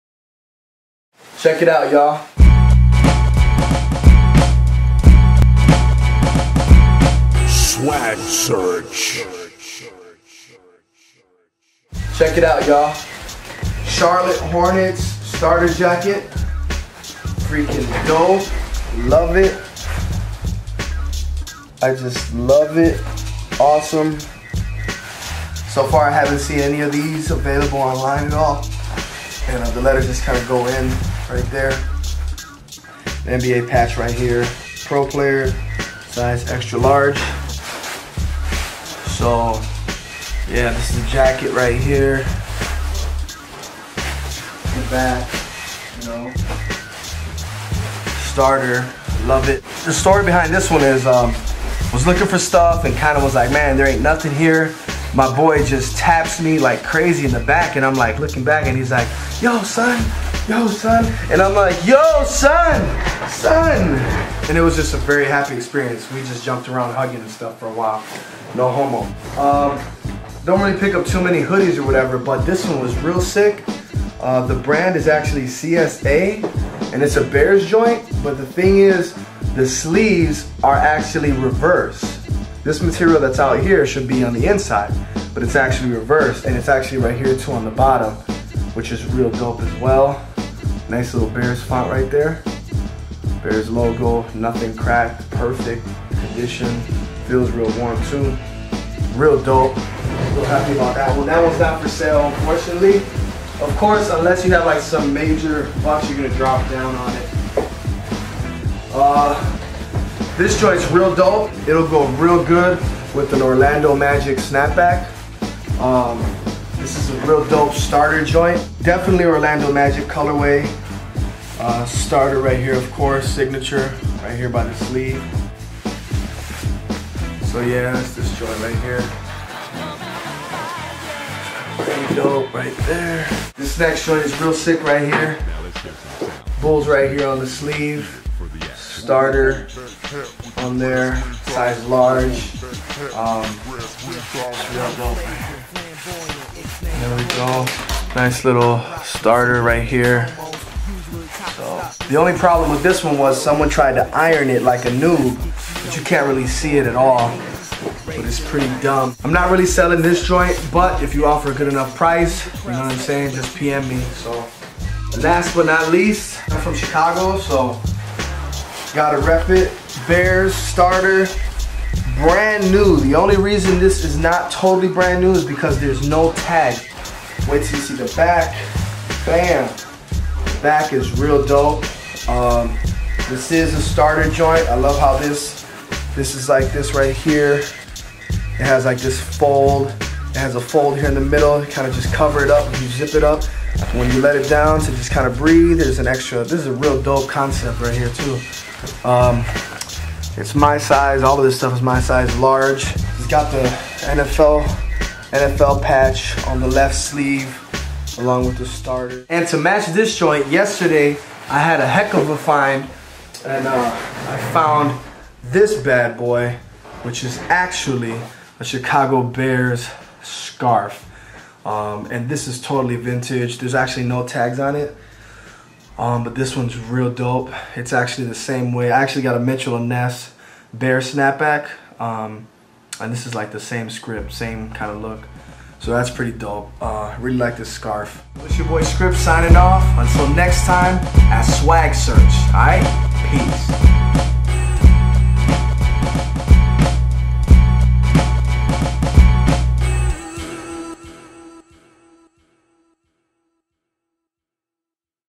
More Pickups for November #1 Starter Jackets & More!
Some November pickups

For Sale Items (displayed at end of video):
New England Patriots Starter Jacket
1987 Cleveland Browns AFC CHAMPIONS Jersey